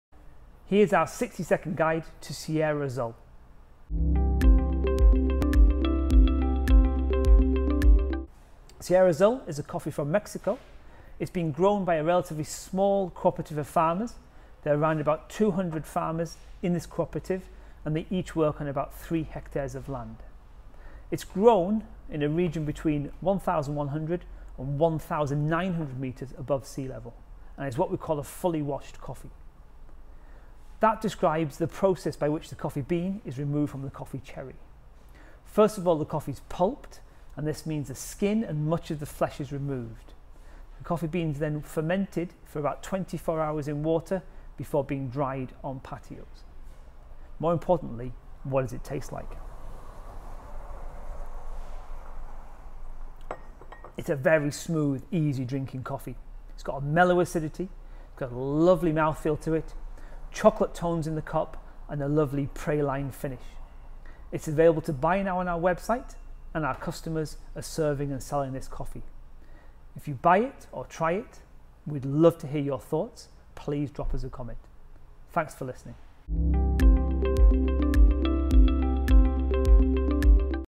Our 60 second guide to Sierra Azul
Our latest coffee from Mexico is Sierra Azul.

Here's the key info about this coffee and what you can expect in the cup